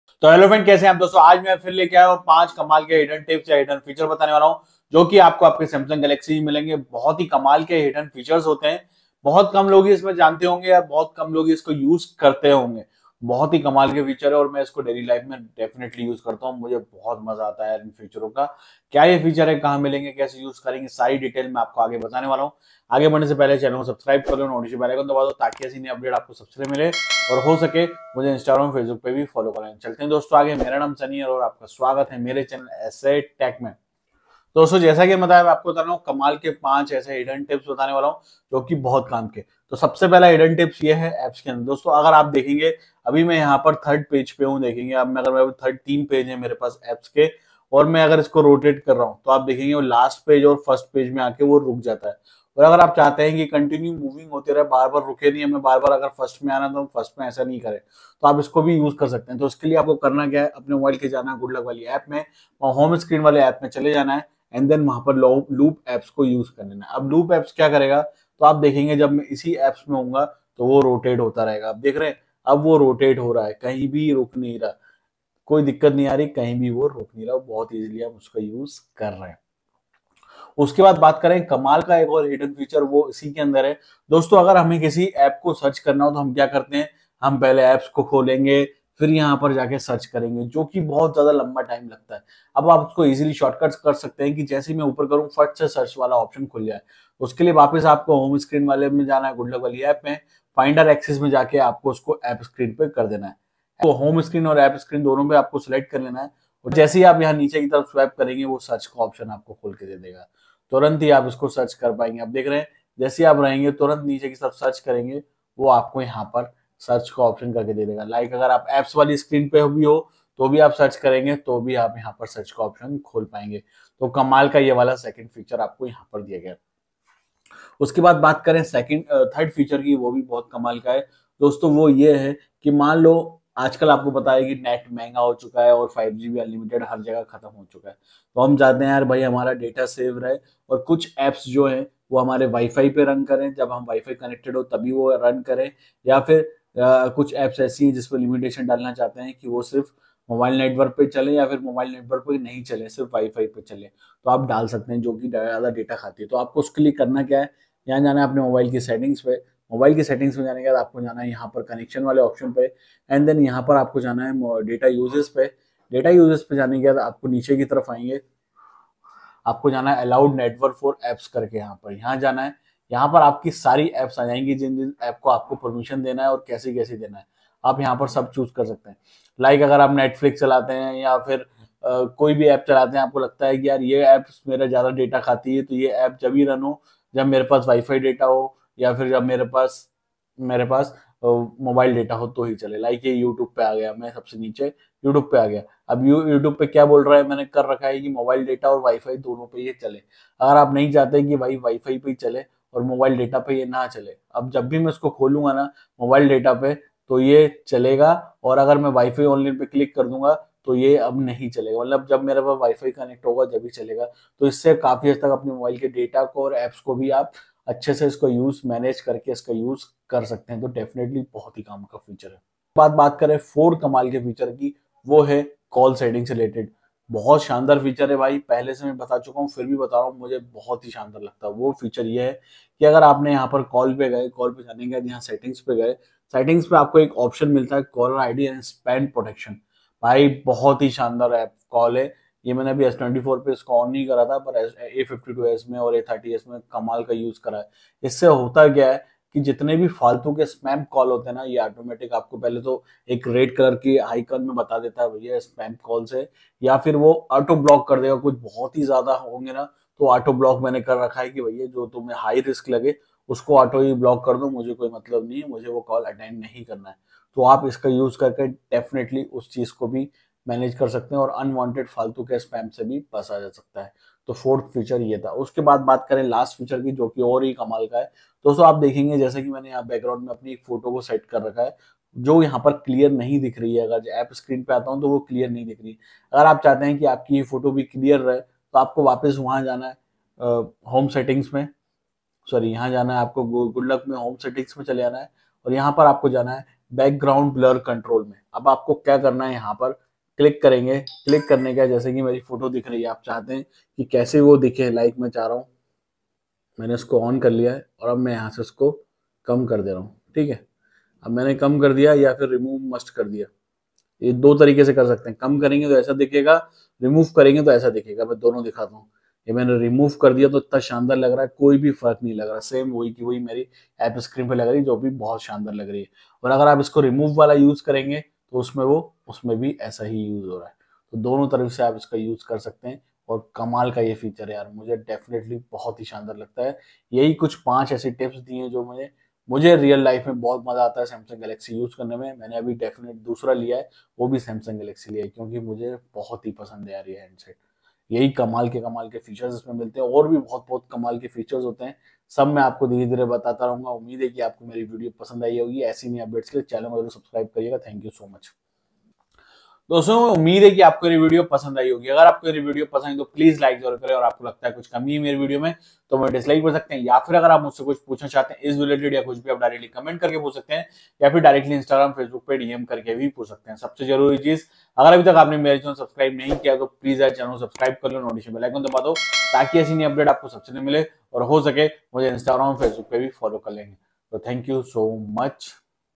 5 Secret Samsung Galaxy Tips You Need to Know
in this video i describe about samsung galaxy 5 hidden tips